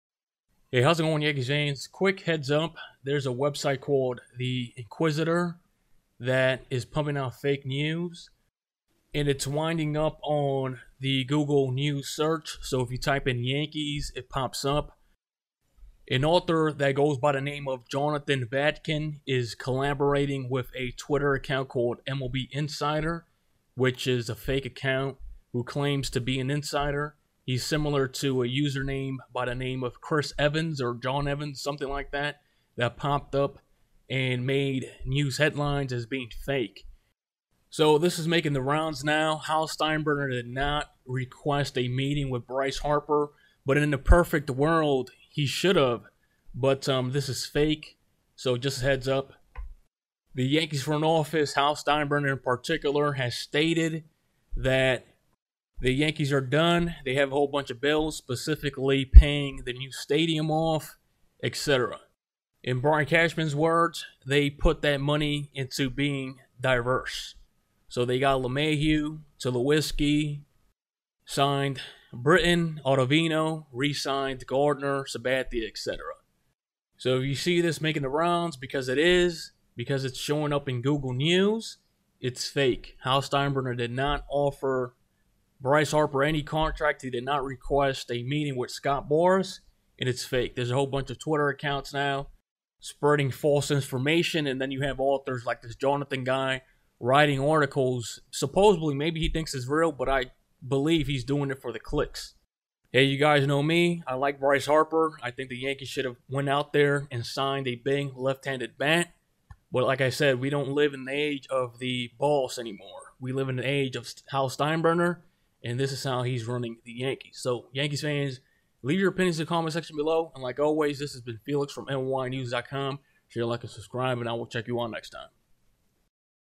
Hal Requested A Meeting with Boras For Bryce Harper?
yankees
mlb
dodgers
scott boras
hal steinbrenner
inquisitr